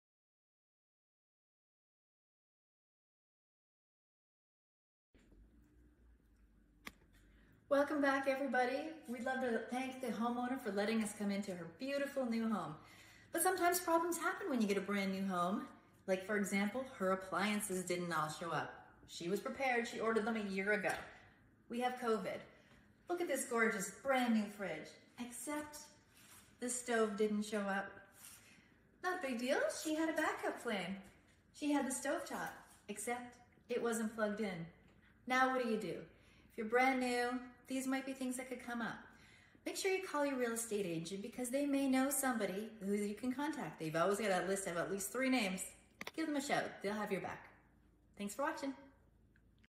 appliances where are they
even your clock uses a chip.  No chips being made means no appliances.  Covid strikes again!!!